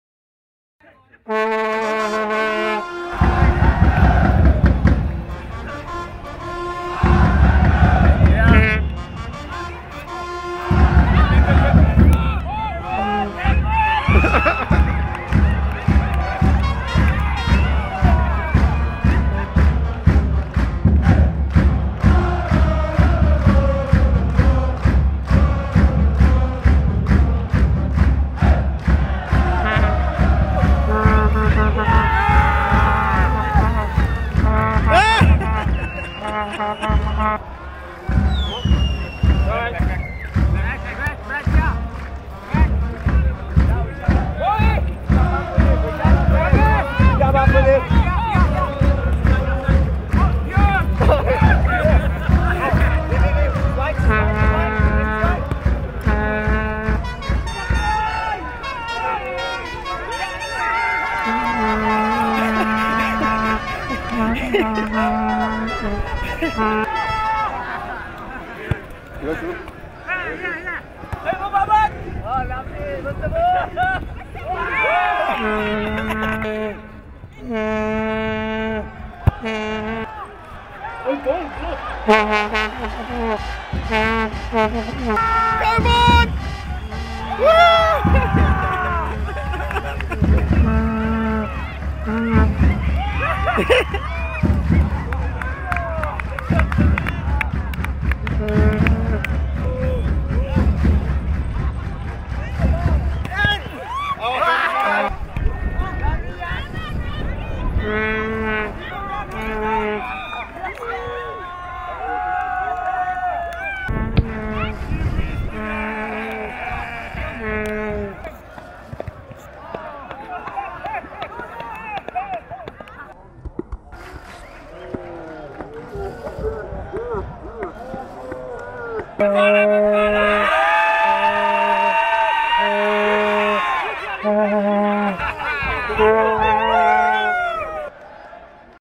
Visual Impact Social Soccer
In the spirit of soccer & the soccer world cup, Visual Impact decided the best way to do some team building was to play some friendly soccer.
The whole team was there, if not playing, then supporting.